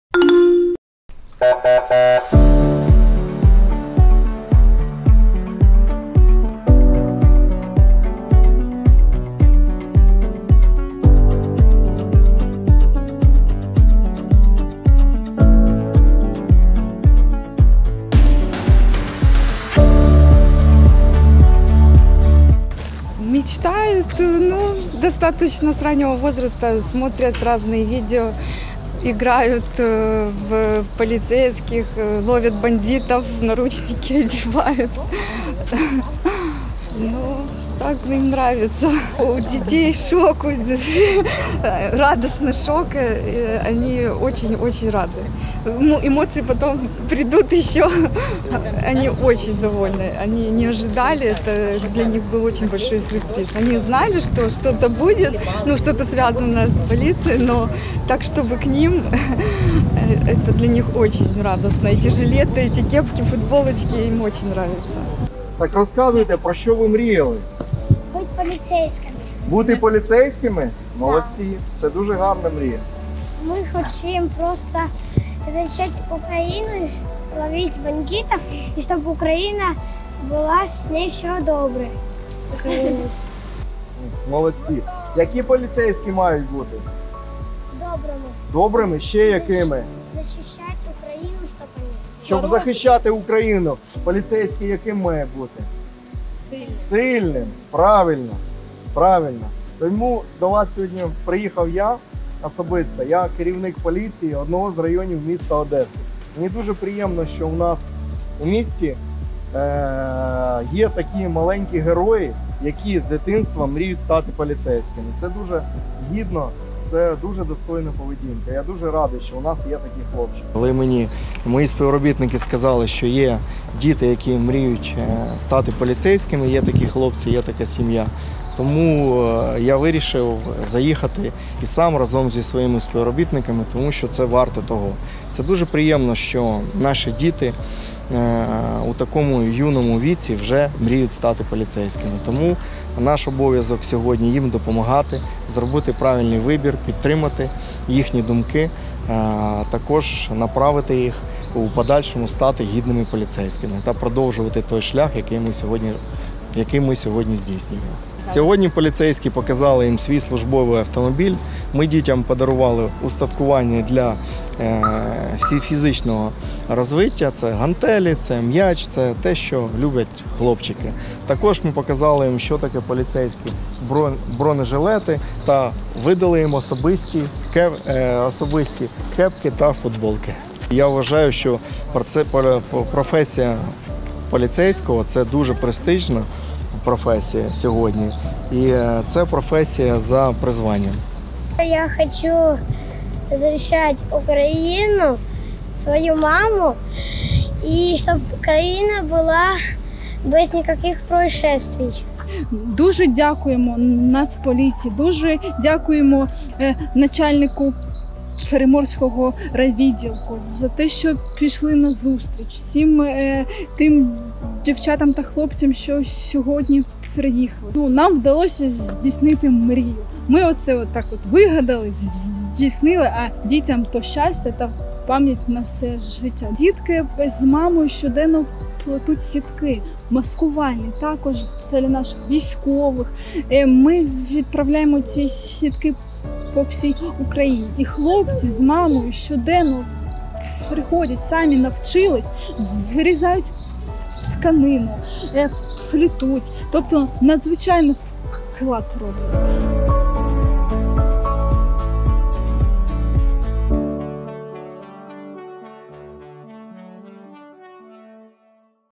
Поліцейські з волонтерами втілили в життя мрію маленьких одеситів
Янчик з Валерієм разом із мамою кожного дня плетуть маскіровочні сітки в одному із волонтерських центрів Одеси. Хлопці завжди мріяли стати поліцейськими, і ось сьогодні їм мрія здійснилася.